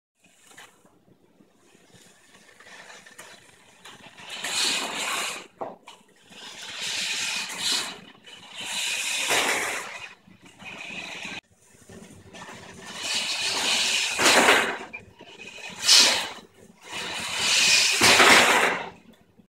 BLUEsat OWR Rover - Version 2.0 (SNEAK PEEK)
The newly constructed Mars Rover climbing obstacles - a sneek peak of footage to come!